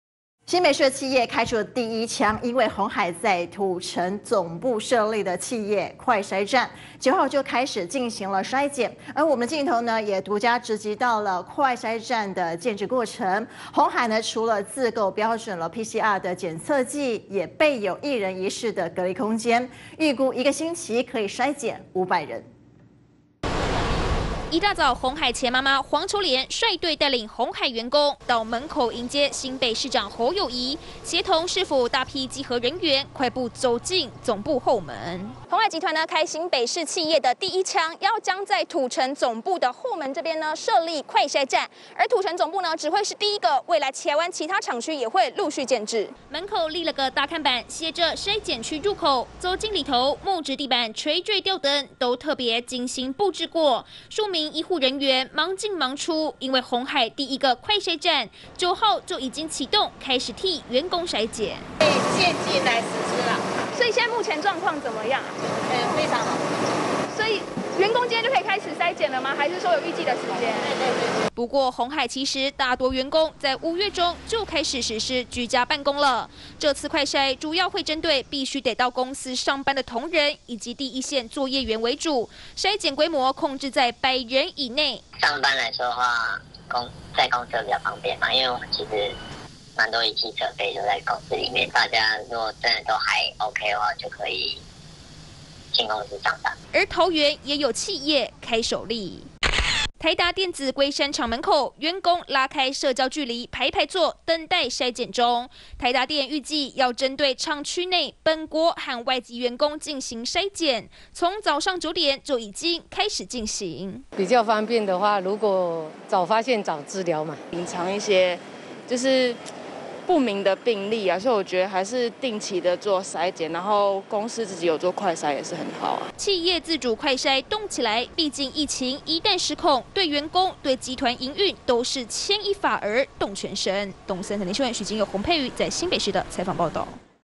直擊鴻海快篩站！ 自備PCR、單獨隔離室
鴻海在土城總部設立的企業快篩站，9日已經開始替員工篩檢了，我們也獨家捕捉到了快篩站建置過程，鴻海除了自購標準的PCR的檢測劑，也備有一人一室的隔離空間，估計一周的篩檢量能可達5百人。

▲ 鎖定【東森亞洲新聞】 周一至週五18:00 ~ 20:00